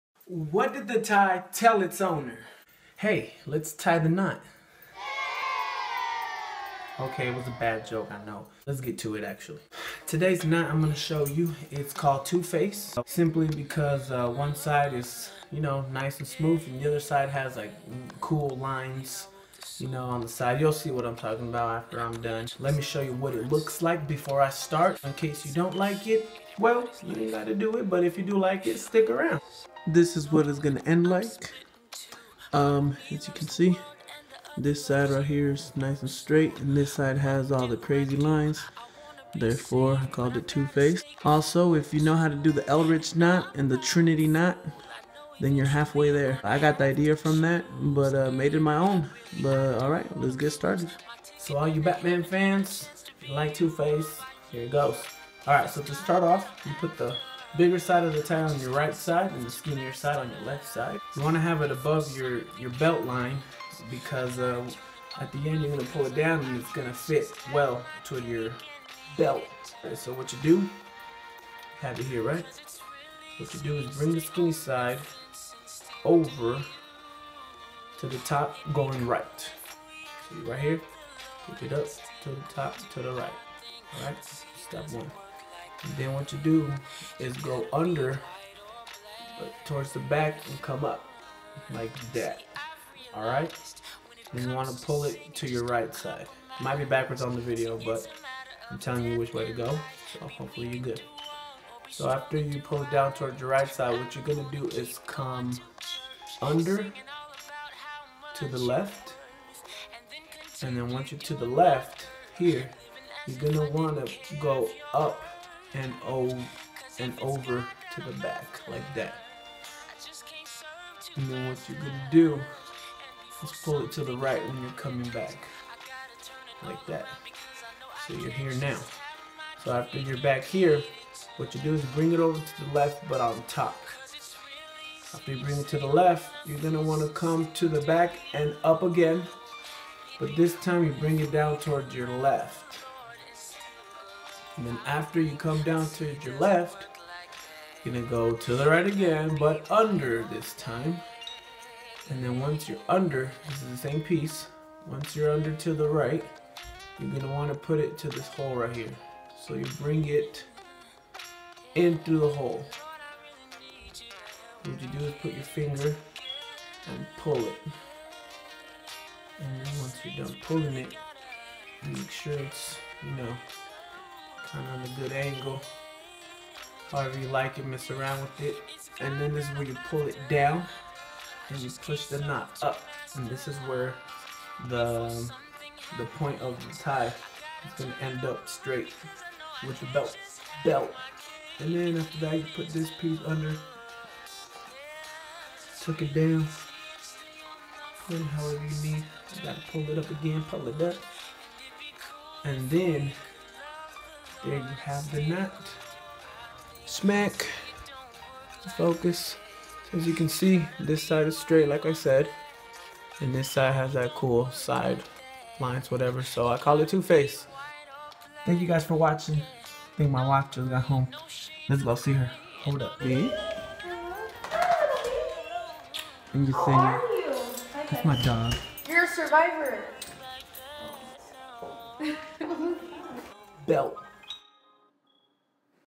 How to tie a tie knot (two face knot)
What's up guys! I made this knot and Sharing with y'all! Share with your daddy's, brothers, cuzns, boyfriends, whatever!! Hope y'all like it!! ✌️